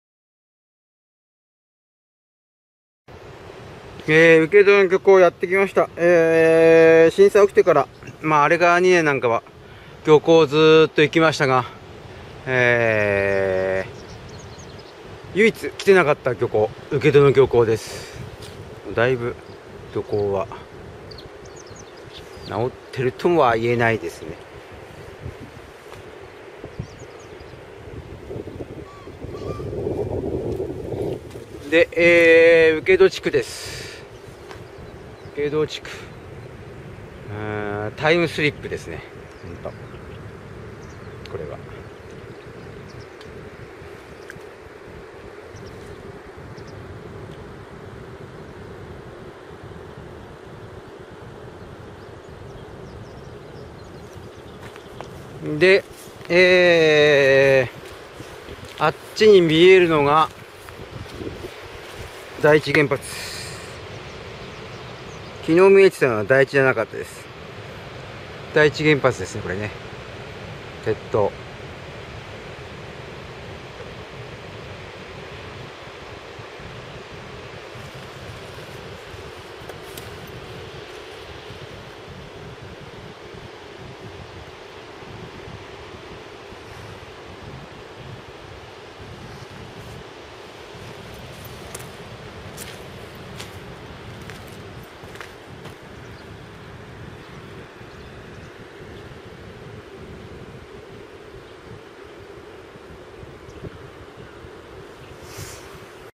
あれから5年と2ヶ月の旅 22 浪江町 請戸魚港
あれから２年の旅で行った、漁港の旅、ニコ動、ユーチューブの前アカウントで、１個づつ紹介したが、その最後、浪江町　請戸漁港、住民でない自分は、なかなか入ることは出来なかった・・規制解除の前にようやく・・５年・・長い年月、ここで漁港の旅は最後だが、請戸漁港の復興はここから始まり・・